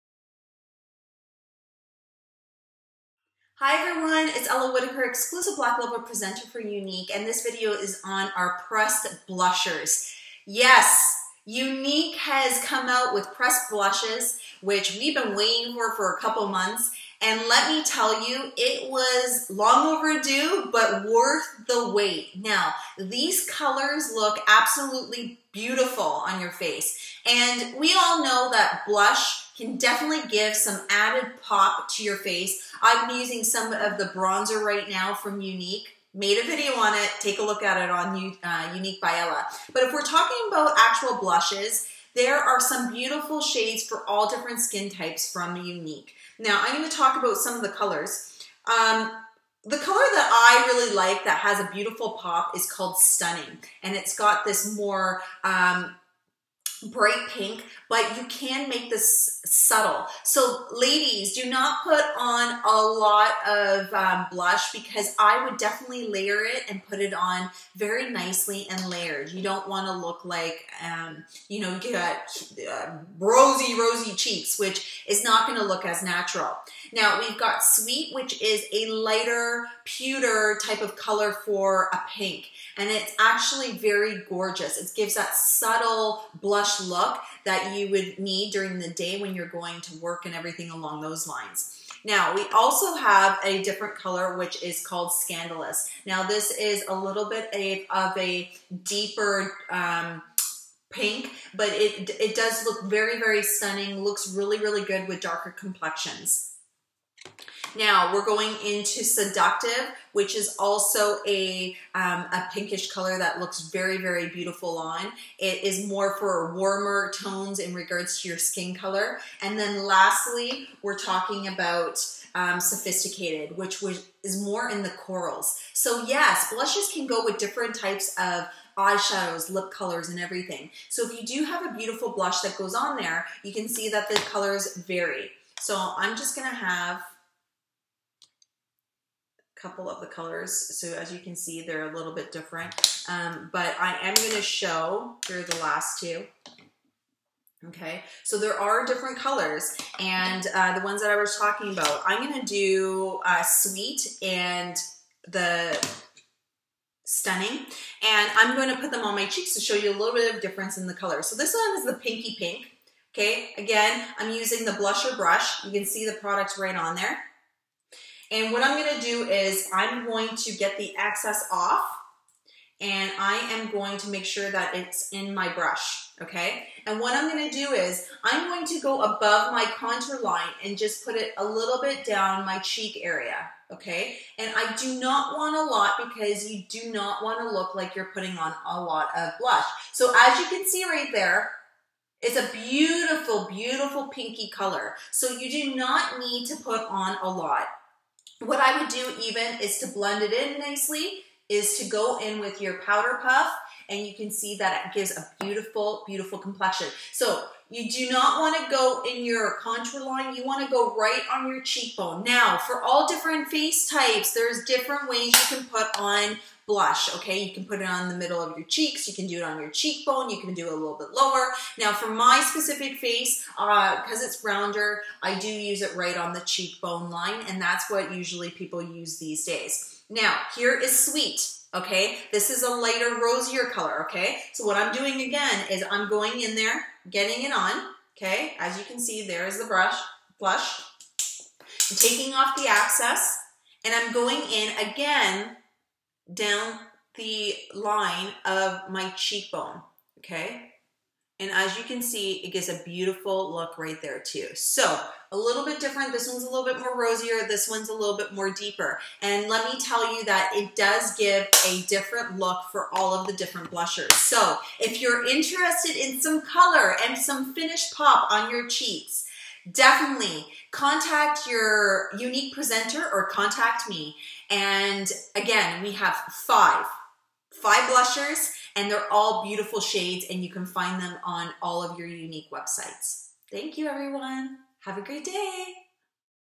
Youniques Pressed Blushers Review
5 stunnings colour from Younique.  If you love blushes, you need to get Younique's!